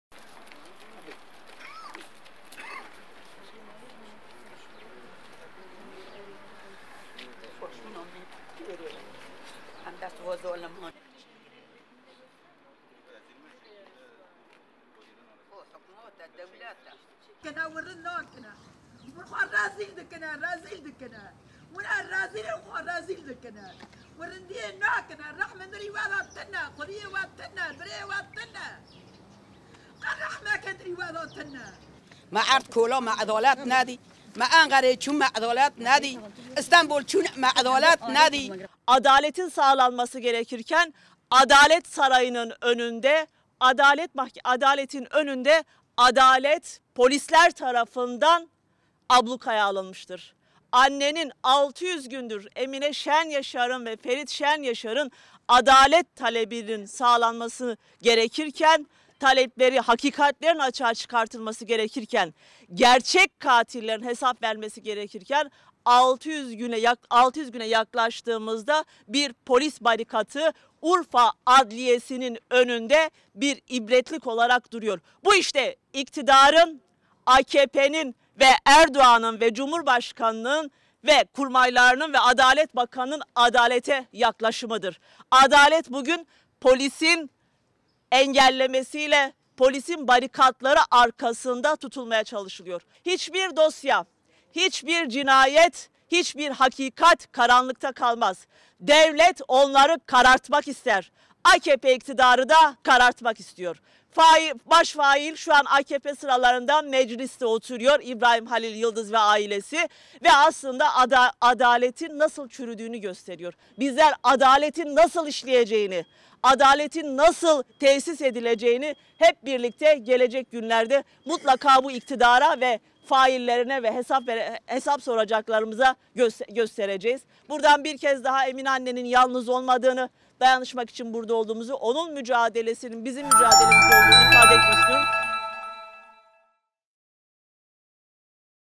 Şenyaşar ailesinin eylemi sürüyor: Fail şu an Meclis’te
Adalet mücadelesini sürdüren Şenyaşar ailesini ziyaret eden HDP Parti Sözcüsü Ebru Günay, “Katliamın baş fail şuan Meclis’te AKP saflarında oturuyor. Polisler aileyi gözaltına alacağına, vekilden, katillerden hesap sorsun” dedi.